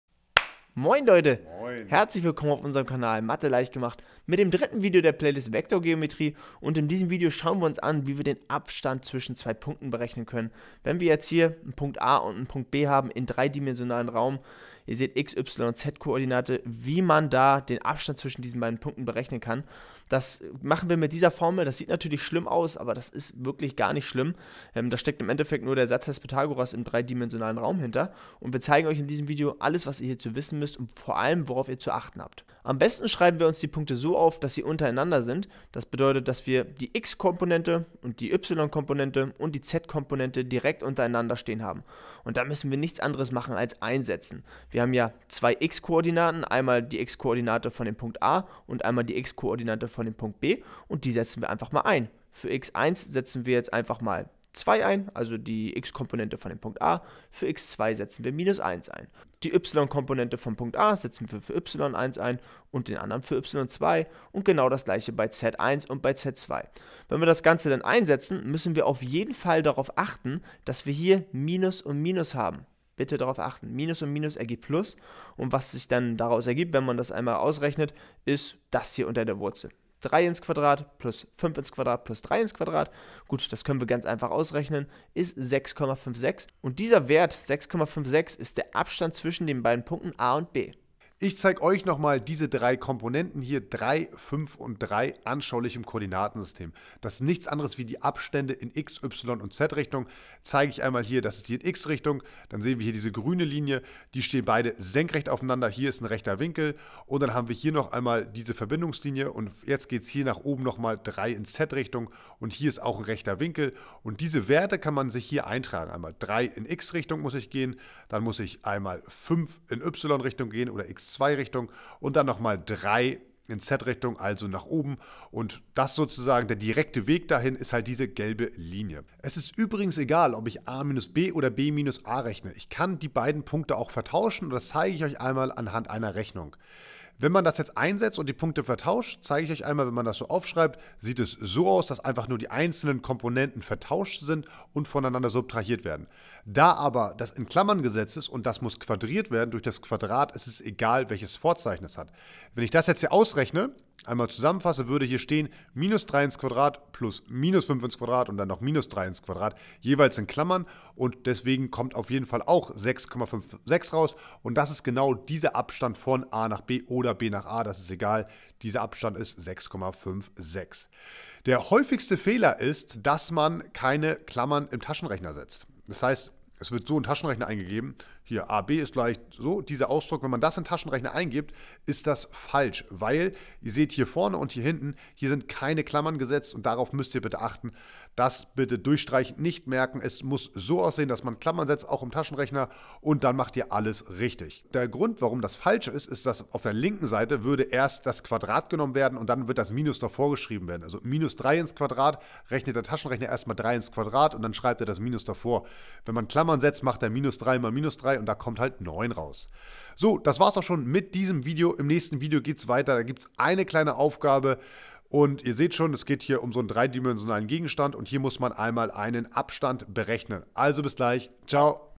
Abstand zwischen zwei Punkten berechnen | 3D-Koordinatensystem | Koordinatengeometrie #3
Drittes Video der Playlist. Wir zeigen, wie du einen Abstand zwischen zwei Punkten berechnen kannst. Hierfür nutzen wir im Endeffekt den Satz des Pythagoras im dreidimensionalen Raum.

0:00 Begrüßung
0:33 Rechnerisch
1:31 Anschaulich
2:09 Reihenfolge der Punkte ist egal
2:50 Häufigster Fehler